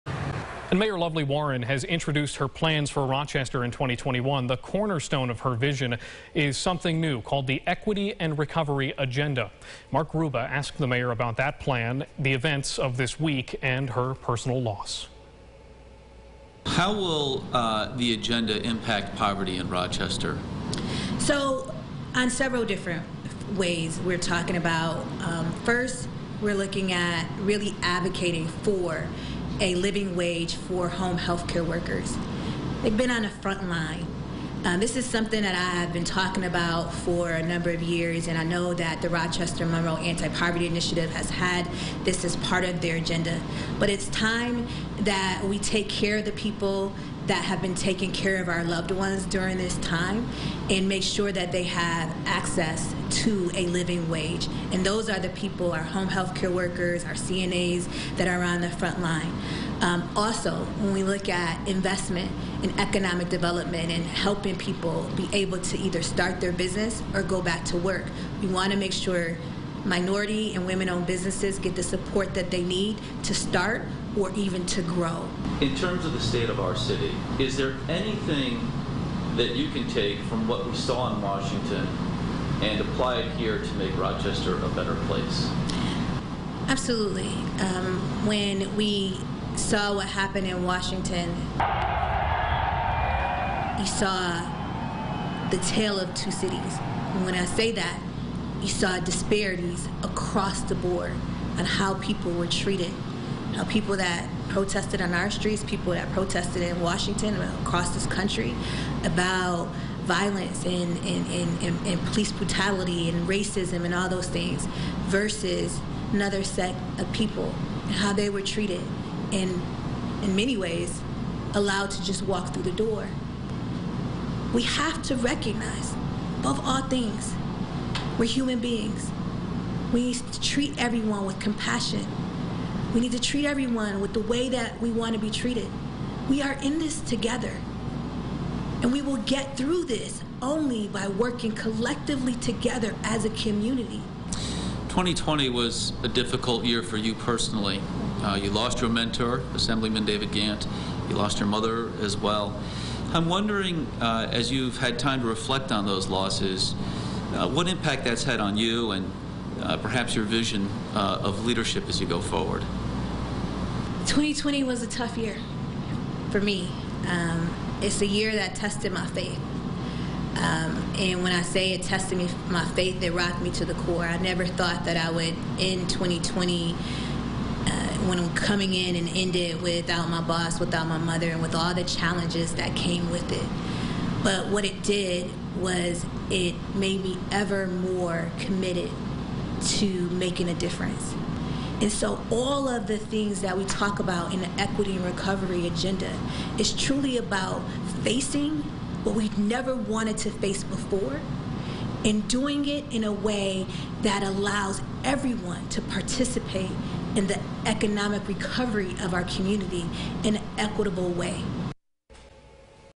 Mark Gruba interviews Mayor Warren
News 8's Mark Gruba interviews Mayor Warren.